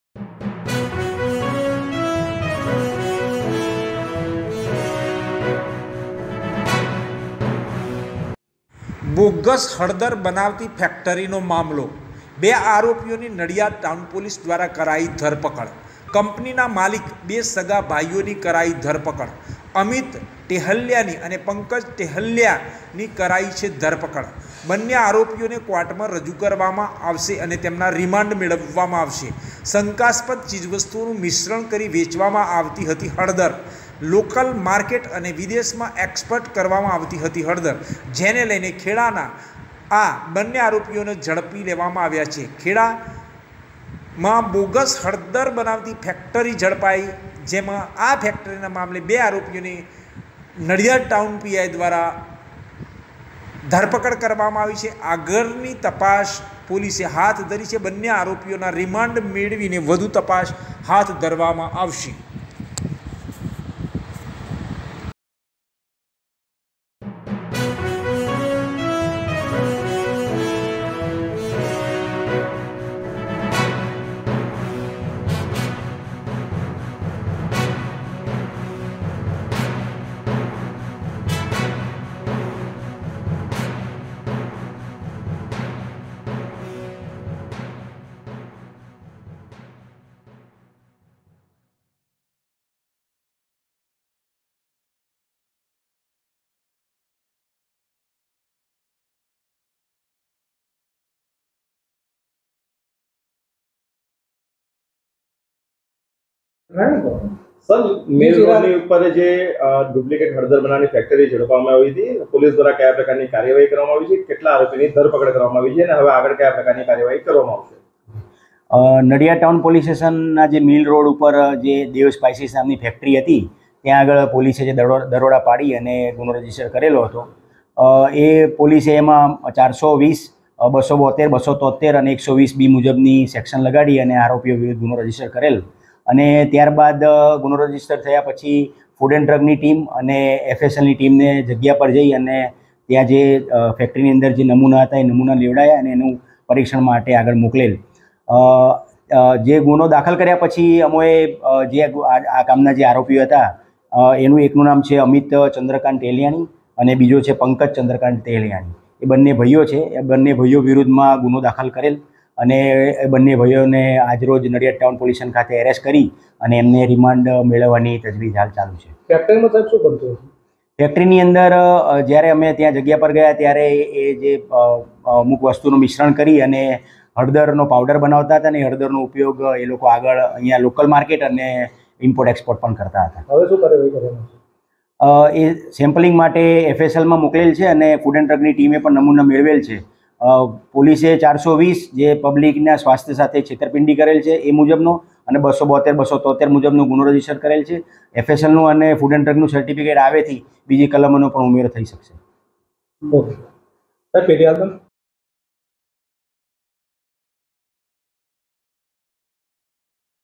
ખેડા બોગસ હળદર બનવતી ફેકટરીનો મામલોબે આરોપીઓની નડીઆદ ટાઉન પોલીસે કરી ધરપકડ
𝗟𝗮𝘁𝗲𝘀𝘁 𝗡𝗲𝘄𝘀: અમિત ટહેલ્યાની અને પંકજ ટહેલ્યાનીની કરાઈ ધરપકડ

બન્ને આરોપીઓને કોર્ટમાં રજૂ કરી માંગવામાં આવશે રિમાન્ડ

શંકાસ્પદ ચીજ વસ્તુઓ મિશ્રણ કરી વેચવામાં આવતી હતી હળદર

લોકલ માર્કેટ અને વિદેશમાં એક્સપોર્ટ કરવામાં આવતી હતી હળદર
બોગસ હળદર બનવતી ફેકટરીનો મામલો

બે આરોપીઓની નડીઆદ ટાઉન પોલીસે કરી ધરપકડ

કંપનીના માલિક બે સગા ભાઈઓની કરાઈ ધરપકડ